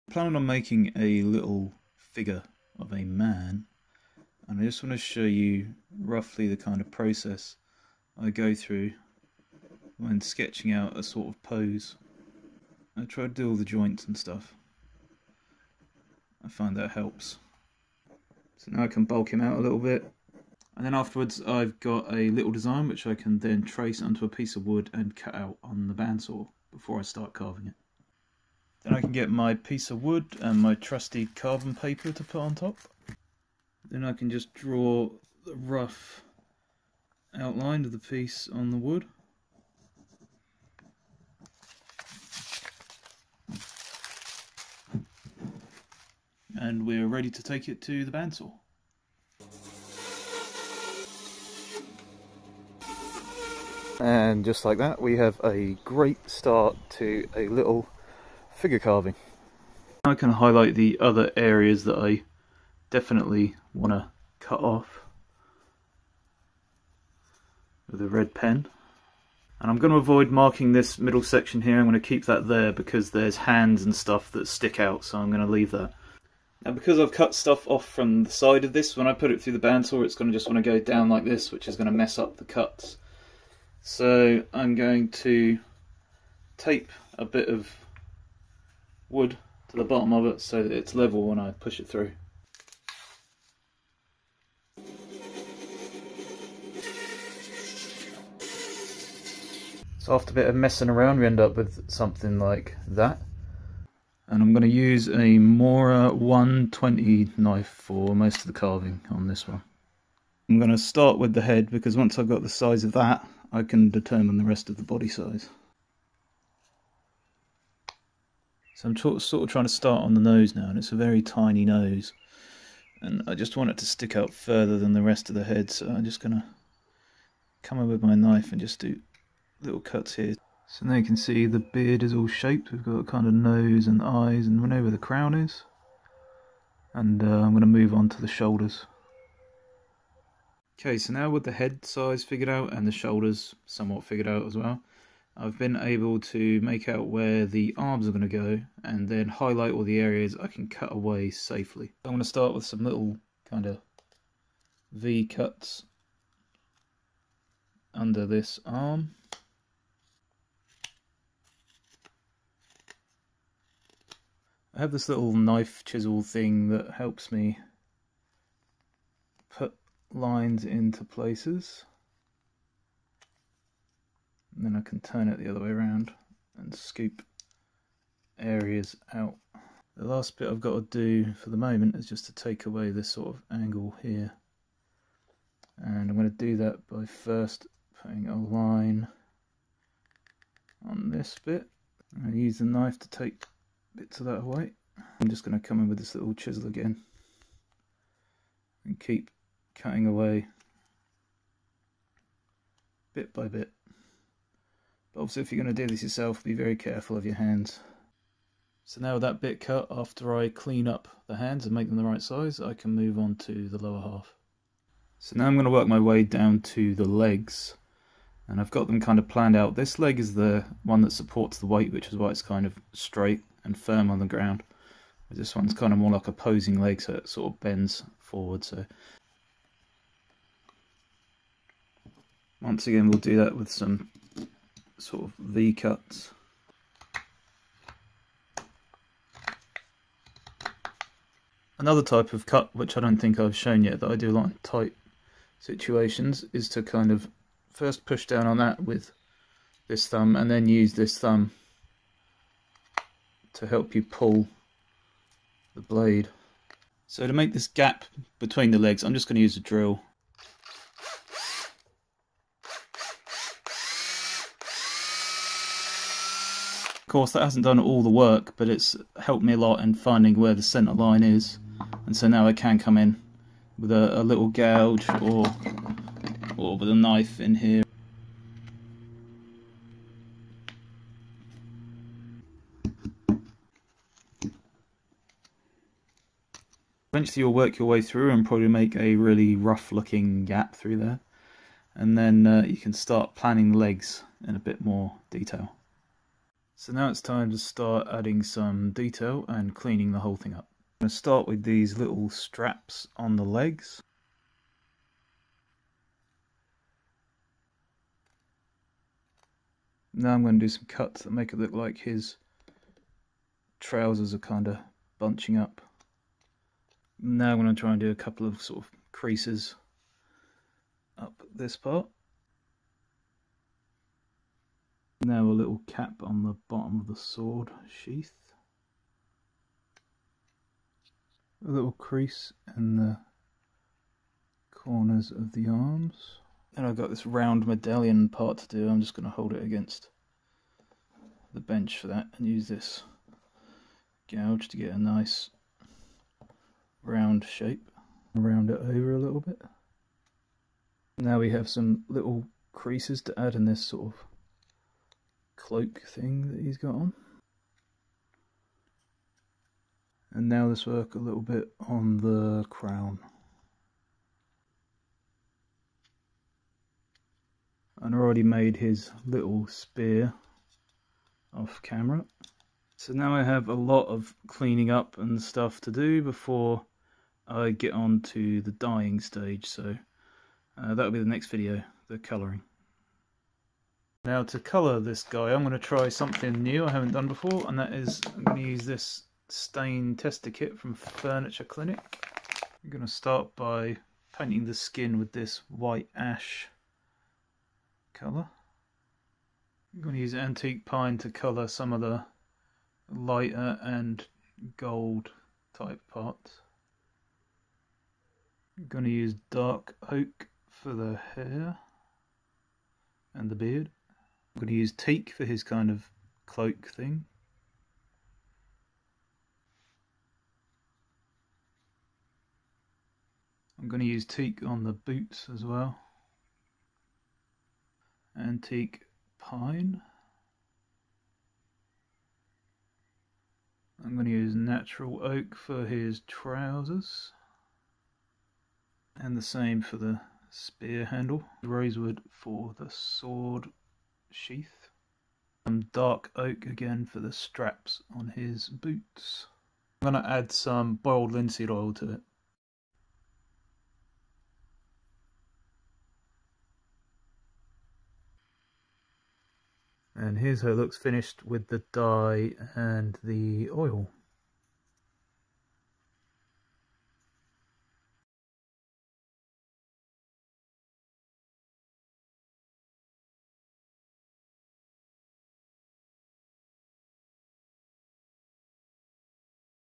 The Process of Carving a Figure - King Edmund of East Anglia
Have you ever looked at a statue or sculpture and just wondered how the design came about or even how it's possible to make a lump of wood into a recognisable figure?

I did a small wood carving series on Instagram recently and I've decided to share it here too, since a few people were interested it.

Thank you for viewing, check out the various links below if you'd like to see what else I get up to.